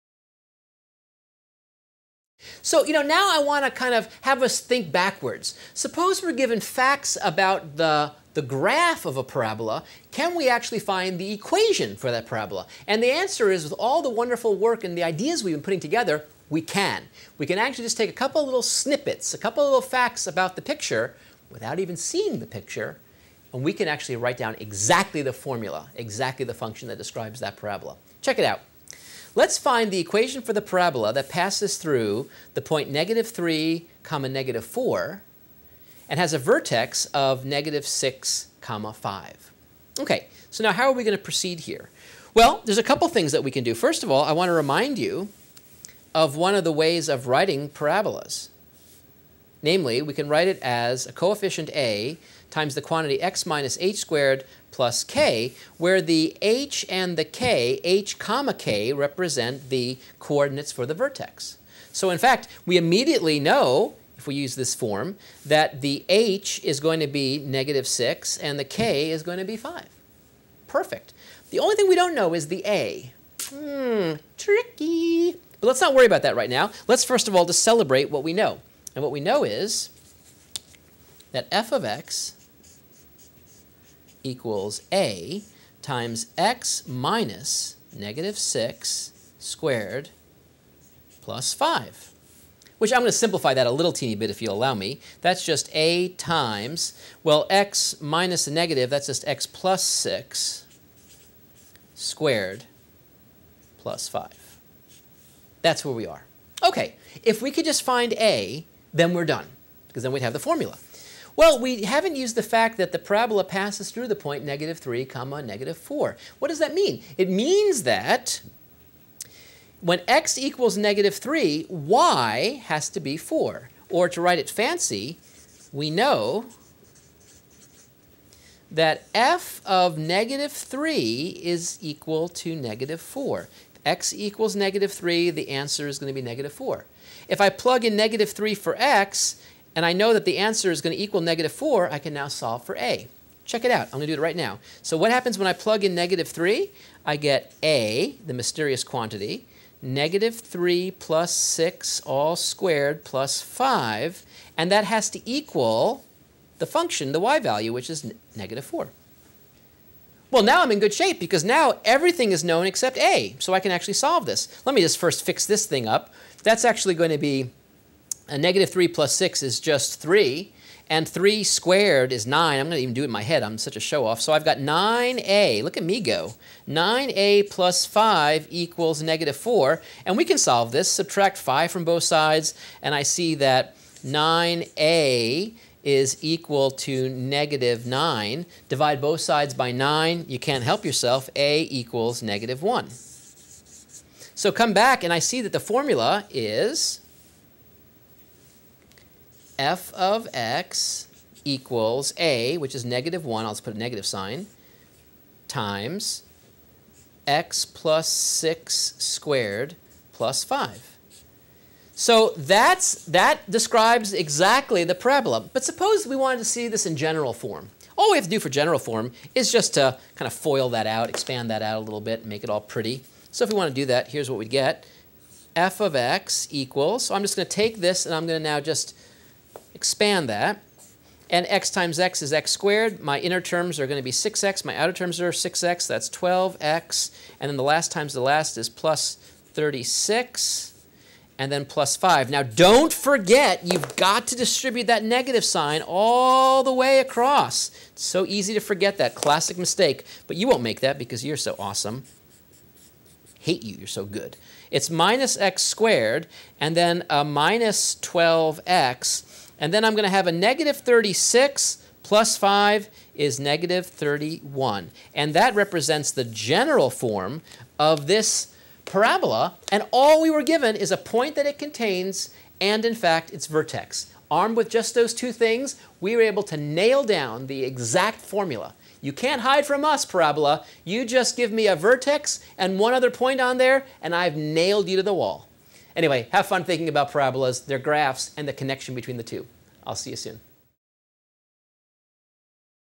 Writing the Equation of a Quadratic Function
Chapter 4 Polynomial Functions, Subchapter 4.1 Quadratic Functions and Models